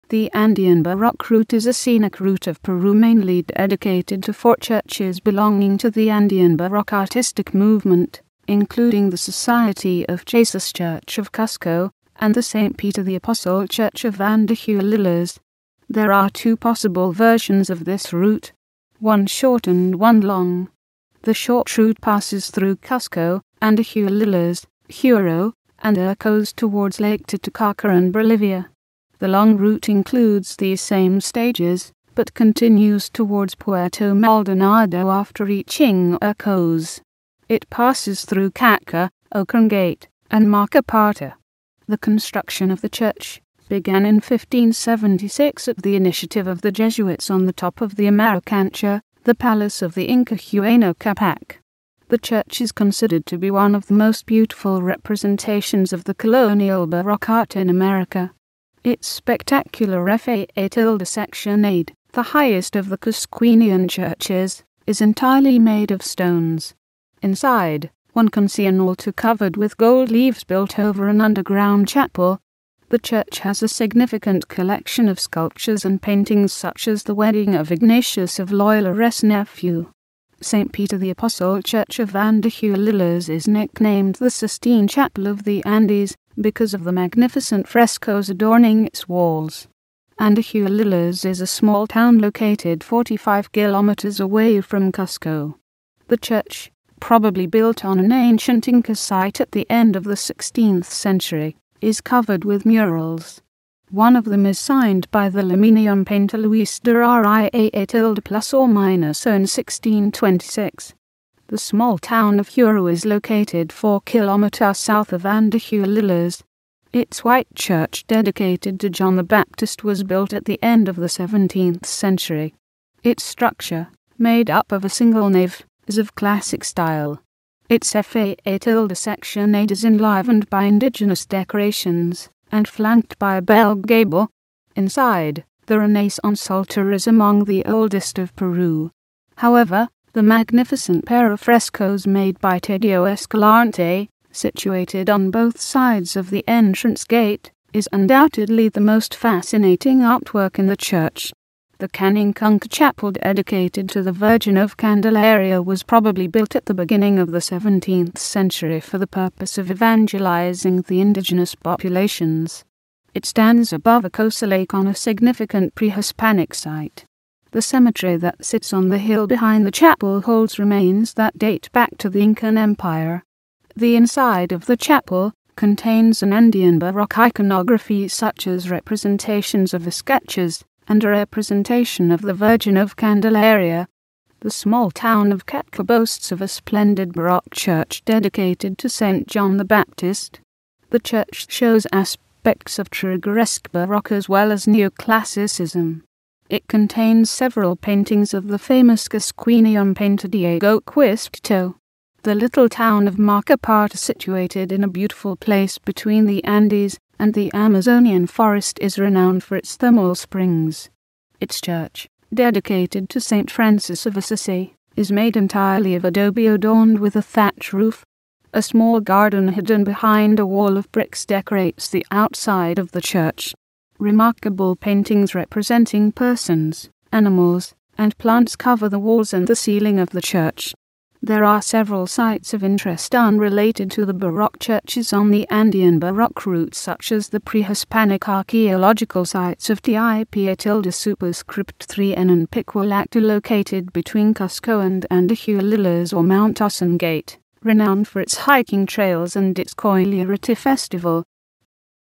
Andean Baroque Route
The Andean Baroque Route is a scenic route of Peru mainly dedicated to 4 churches belonging to the Andean Baroque artistic movement, including the Society of Jesus Church of Cusco and the Saint Peter the Apostle Church of Andahuaylillas.

There are two possible versions of this route: one short and one long. The short route passes through Cusco, Andahuaylillas, Huaro and Urcos towards lake Titicaca and Bolivia. The long route includes these same stages but continues towards Puerto Maldonado after reaching Urcos. It passes through Ccatcca, Ocongate and Marcapata.

The construction of the church began in 1576 at the initiative of the jesuits on the top of the Amarucancha, the Palace of the Inca Huayna Capac. The church is considered to be one of the most beautiful representations of the colonial baroque art in America. Its spectacular façade, the highest of the Cusquenian churches, is entirely made of stones. Inside, one can see an altar covered with gold leaves built over an underground chapel. The church has a significant collection of sculptures and paintings such as 'the wedding of Ignatius of Loyola’s nephew'.

Saint Peter the Apostle Church of Andahuaylillas is nicknamed the Sistine Chapel of the Andes because of the magnificent frescos adorning its walls. Andahuaylillas is a small town located 45 km away from Cusco. The church, probably built on an ancient Inca site at the end of the 16th century, is covered with murals. One of them is signed by the Limenian painter Luis de Riaño in 1626.

The small town of Huaro is located 4 km south of Andahuaylillas. Its white church dedicated to John the Baptist was built at the end of the 17th century. Its structure, made up of a single nave, is of classic style. Its façade is enlivened by indigenous decorations and flanked by a bell gable. Inside, the Renaissance altar is among the oldest of Peru. However, the magnificent pair of frescos made by Tadeo Escalante, situated on both sides of the entrance gate, is undoubtedly the most fascinating artwork in the church.

The Canincunca chapel dedicated to the Virgin of Candelaria was probably built at the beginning of the 17th century for the purpose of evangelizing the indigenous populations. It stands above Urcos´ lake on a significant pre-Hispanic site. The cemetery that sits on the hill behind the chapel holds remains that date back to the Incan Empire. The inside of the chapel contains an Andean Baroque iconography such as representations of viscachas and a representation of the Virgin of Candelaria.

The small town of Ccatcca boasts of a splendid baroque church dedicated to Saint John the Baptist.

The church shows aspects of churrigueresque baroque as well as neoclassicism. It contains several paintings of the famous Cusquenian painter Diego Quispe Tito.

The little town of Marcapata situated in a beautiful place between the Andes and the Amazonian forest is renowned for its thermal springs. Its church, dedicated to Saint Francis of Assisi, is made entirely of adobe adorned with a thatch roof. A small garden hidden behind a wall of bricks decorates the outside of the church. Remarkable paintings representing persons, animals, and plants cover the walls and the ceiling of the church.

There are several sites of interest unrelated to the baroque churches on the Andean Baroque Route such as the pre-Hispanic archaeological sites of Tipón and Piquillakta located between Cusco and Andahuaylillas or Mount Ausangate, renowned for its hiking trails and its Qoyllurit'i festival.

licensed with CC Attribution-ShareAlike 3.0. This video is licensed with CC Attribution-Share-Alike 3.0 In order to adapt this content it is required to comply with the license terms.